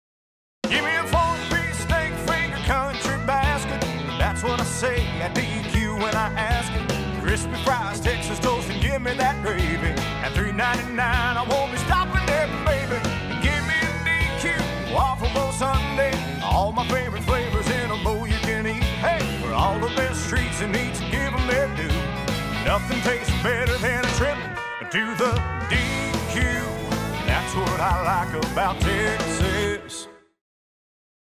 Texas DQ December TV.mov
Nobody serves hot delicious steak fingers like Texas DQ and with our Steak Finger Country Basket, you can get four to six golden, crunchy steak fingers, DQ fries, Texas Toast and the best tasting cream gravy anywhere for one special low price. DQ. That's what I like about Texas.